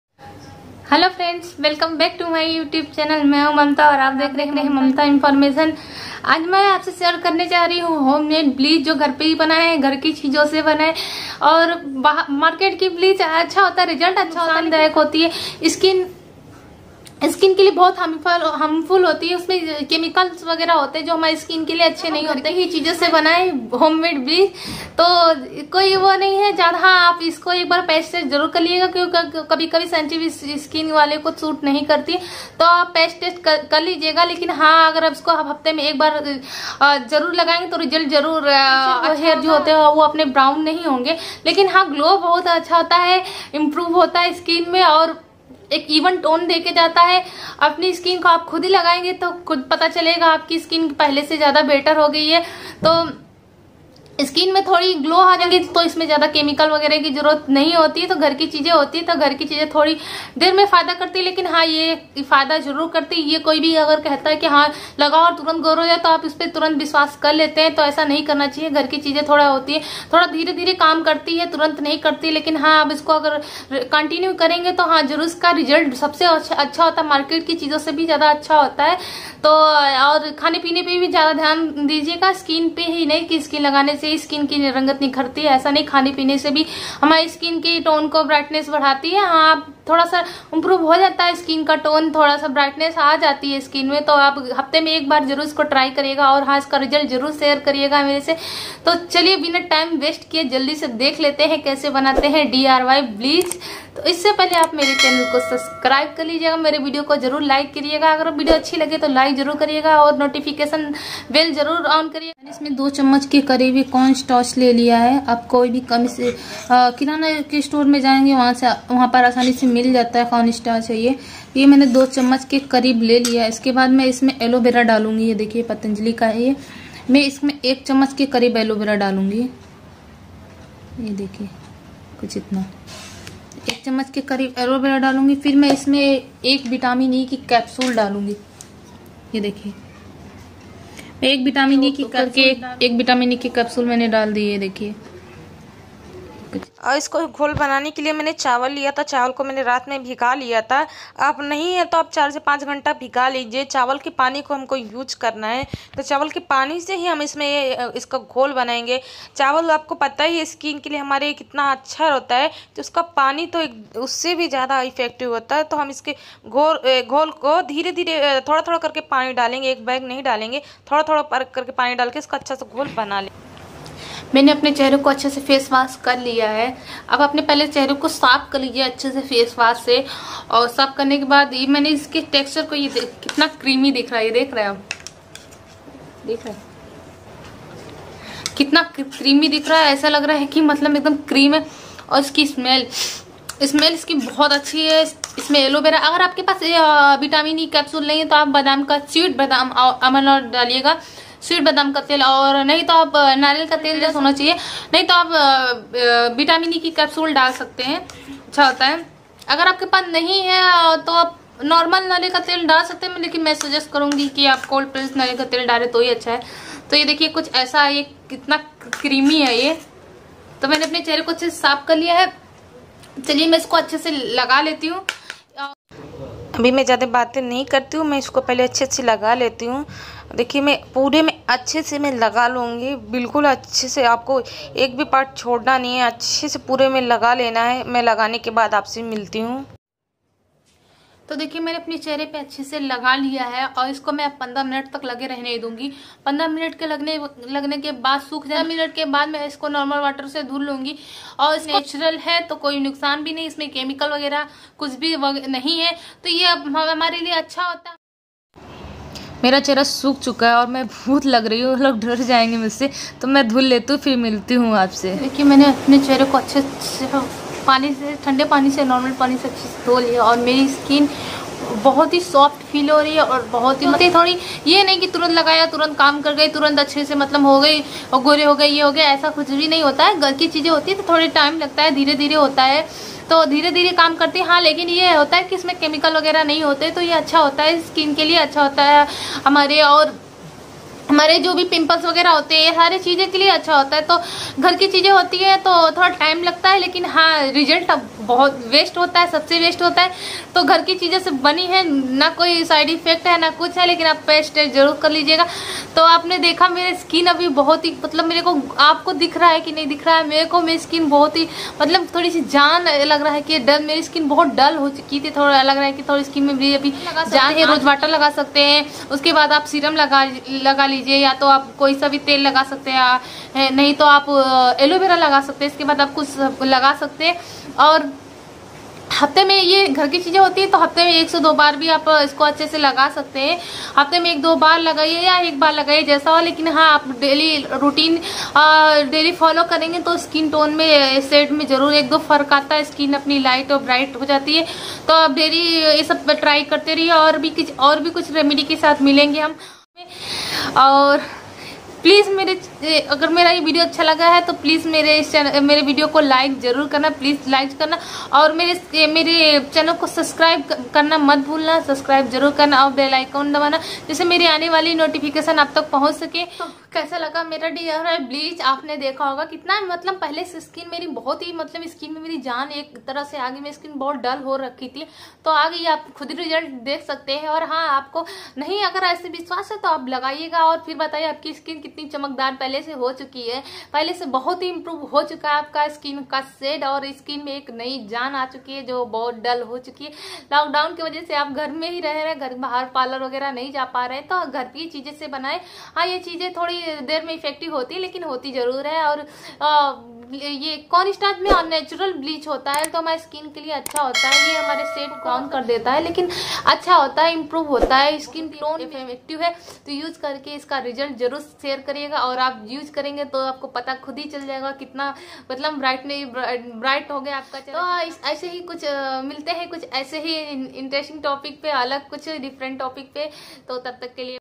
DIY Bleach Rs5,तुरन्त गोरा कर देगा100% Guarantee,Homemade Bleach200%natural remove darkspot,
DIY Bleach At Home Rs5,तुरन्त गोरा कर देगा100% Guarantee,Homemade Bleach rice Water remove Darkspot
Homemade bleach ghar par banaye aur Turant result paaye
Skin very bright and glow 100% natural
Remove dark spot and instan glow
Esko banana Bahut aasan hai market se Khuch lene Ki jarort nhi padegi